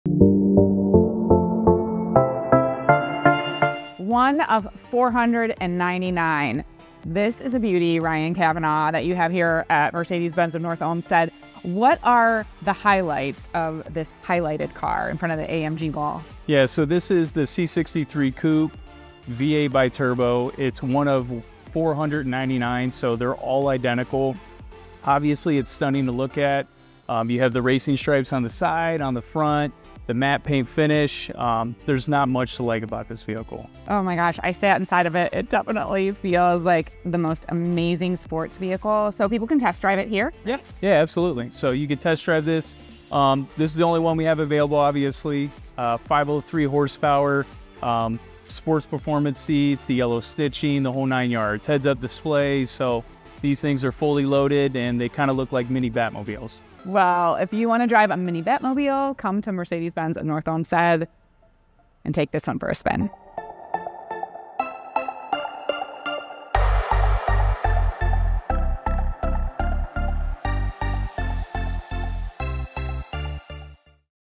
2023 Mercedes AMG C63 S Coupe | Walkaround | Mercedes Benz North Olmsted
Check out this amazing 2023 Mercedes AMG C63 S Coupe Walkaround w Andrea